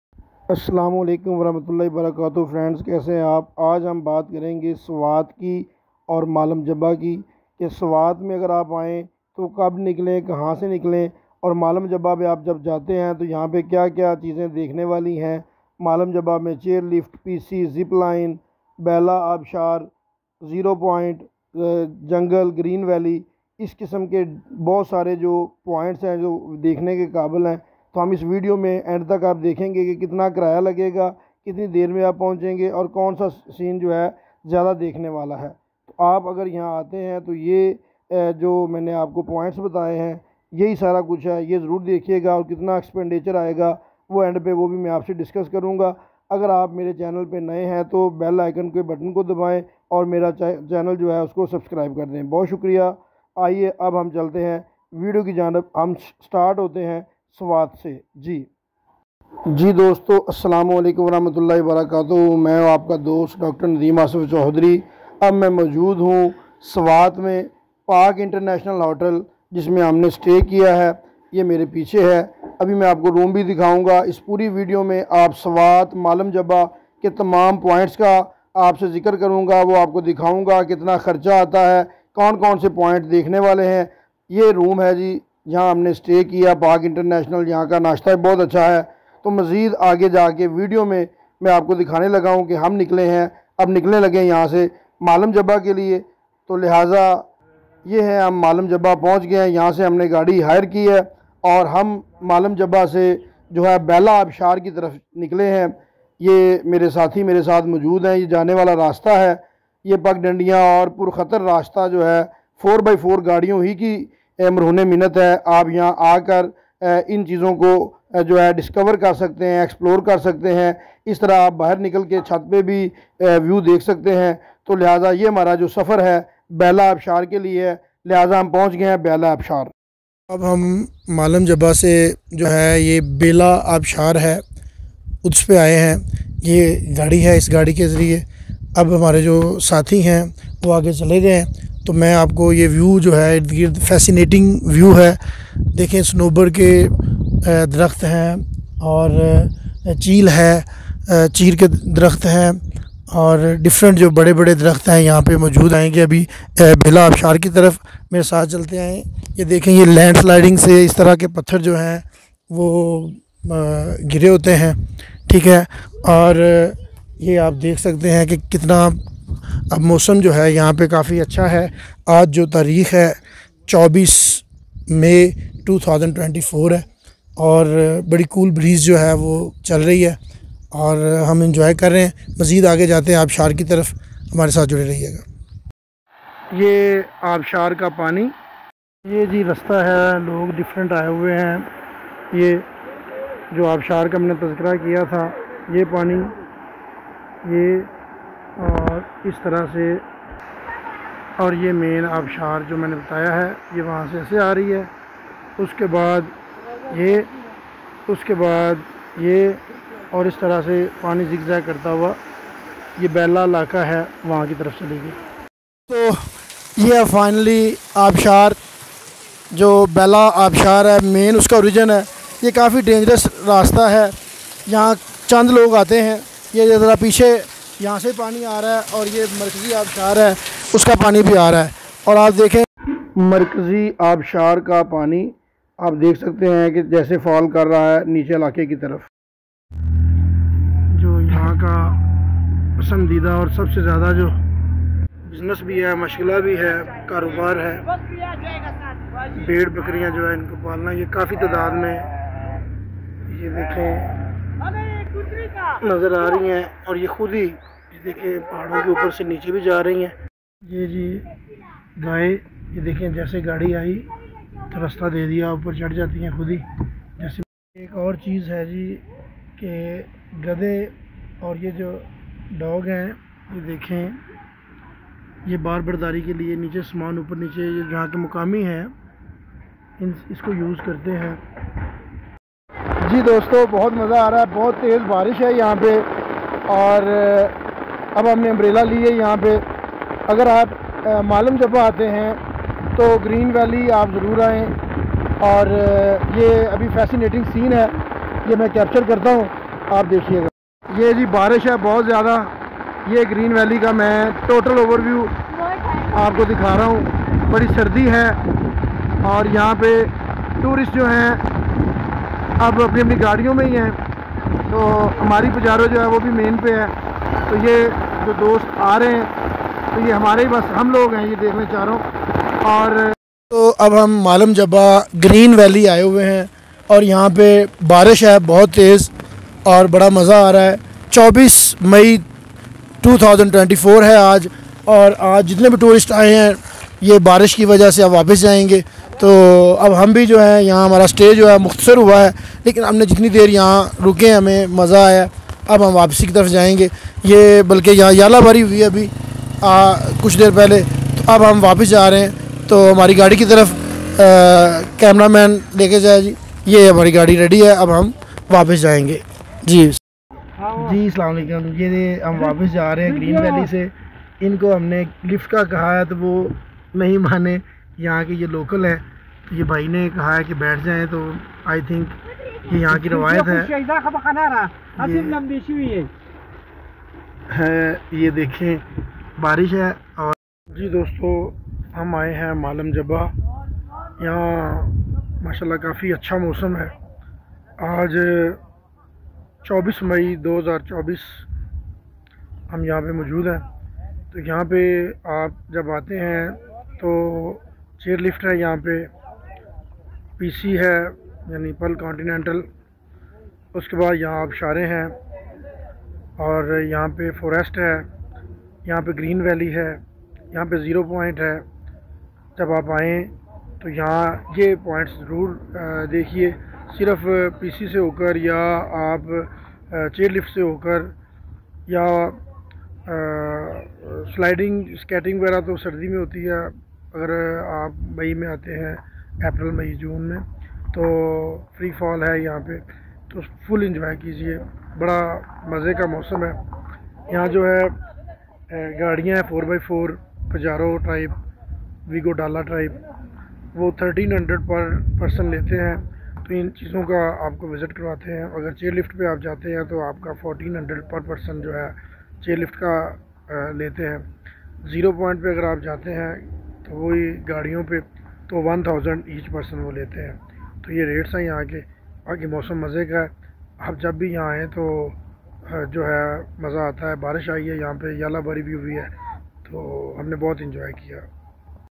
swat valley / malam jabba / latest update malam jabba / visit to malam jabba / visit to swat
It all about swat valley and malam jabba. New updates with present ticket price and expenditure details